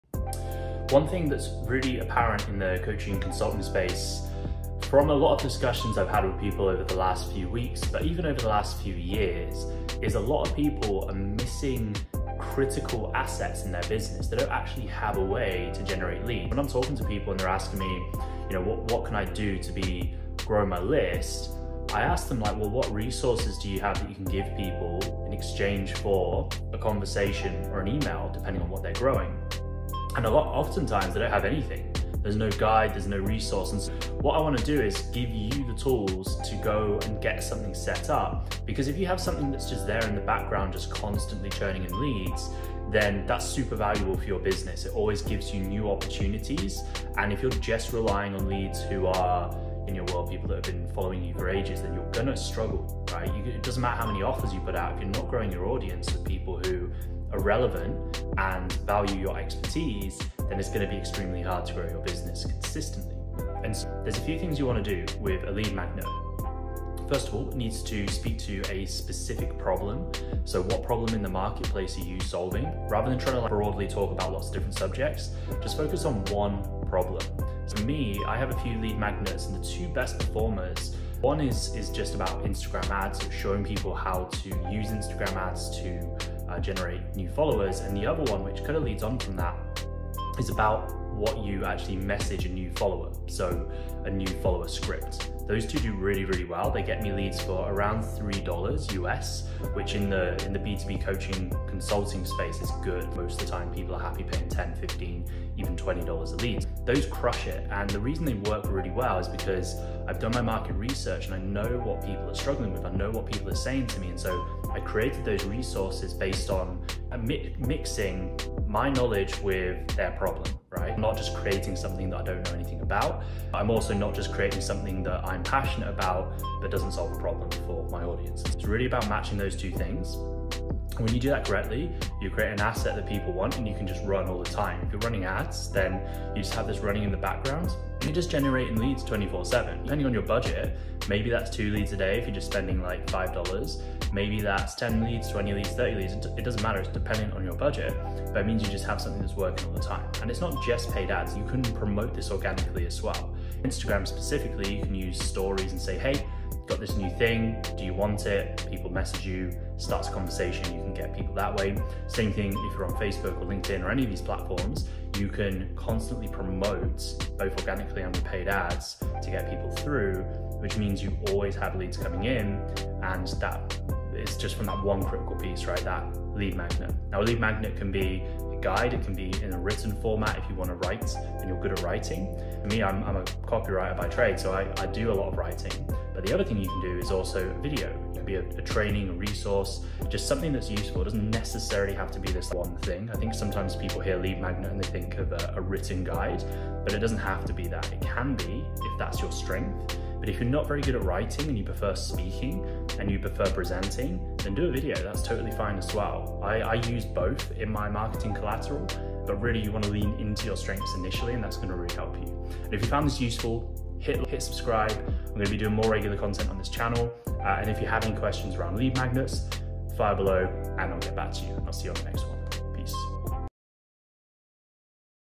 How to create a lead magnet for a coaching or consulting offer
In this video I want to break down a topic I have talked to many coaches and consultants about over the years: Lead magnets.